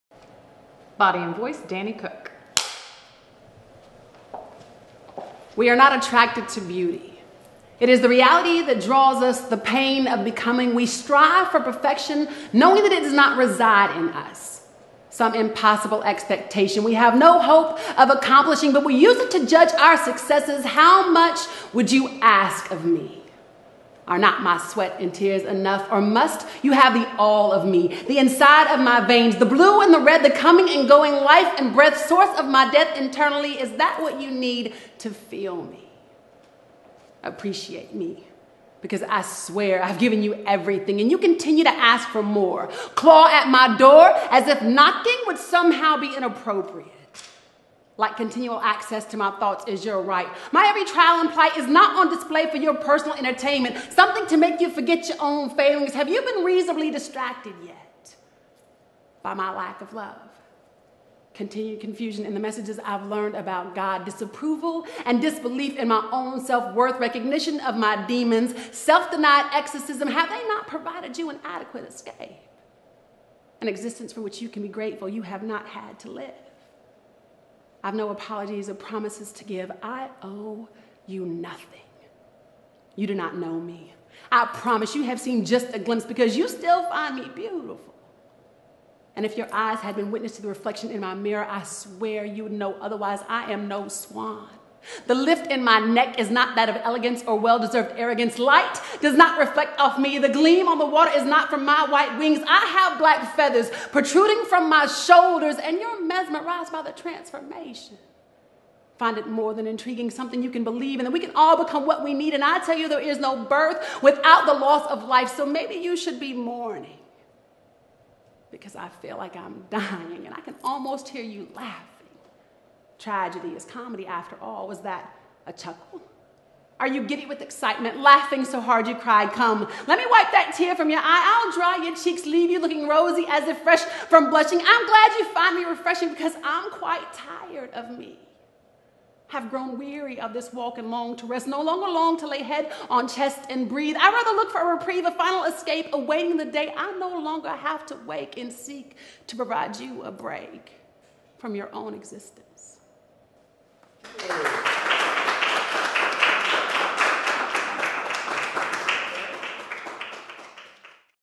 Body & Voice - Dani Cook | Charlotte Video Project
'Body & Voice' is a video series featuring some of Charlotte's best performance artists who use BODY movement and the VOICE as their media (comics, poets and actors). Each artist performs a 3-5 minute piece in a controlled studio environment at smARTlab

Body & Voice - Dani Cook